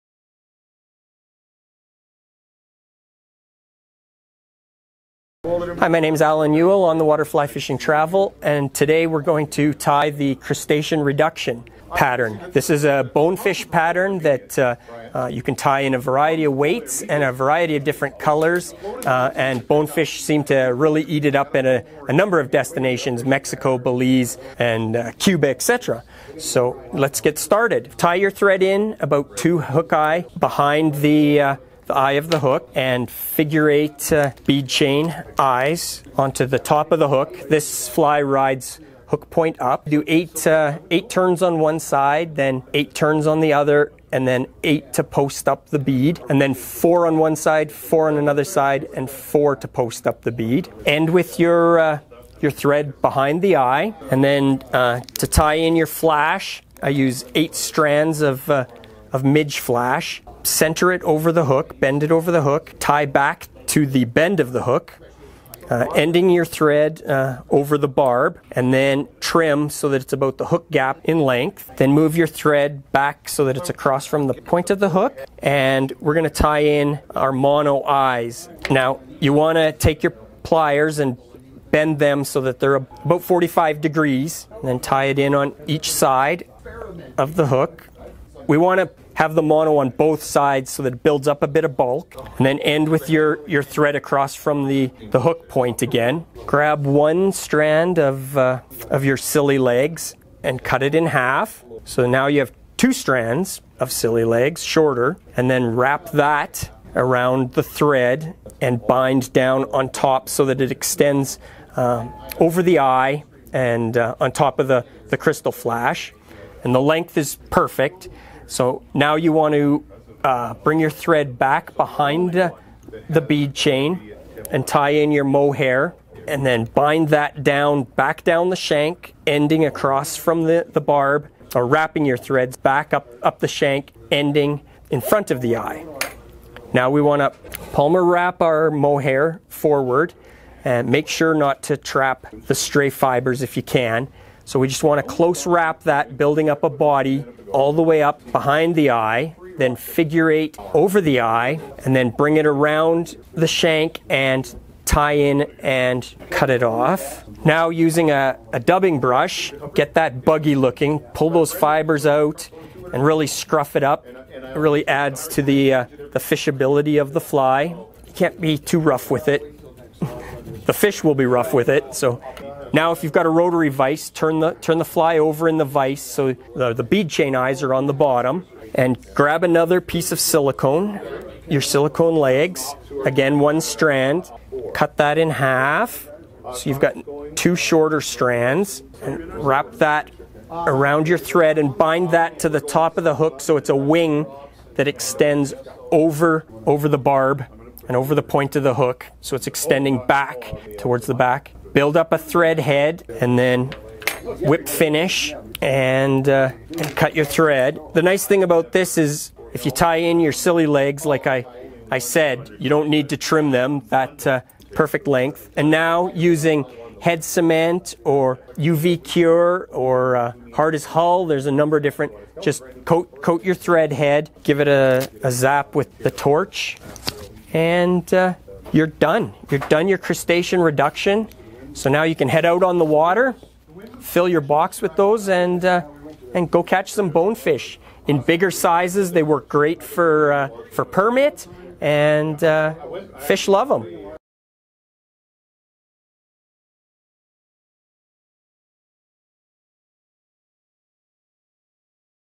Alan Youell from On the Water Fly Fishing Travel ties the Crustacean Reduction.
Alan Youell has traveled extensively throughout the worlds best saltwater destinations and loves sharing those experiences through his outfitting company, On the Water Fly Fishing Travel. Today he is tying one of his go to designs, the Crustacean Reduction, here at Bow River Troutfitters. This is a deadly fly and yet is easy to tie using some basic materials. Try out this effective shrimp pattern before your next trip to the salt. Visit bowrivertroutfitters.com to find all the materials you need to tie this and so many more patterns.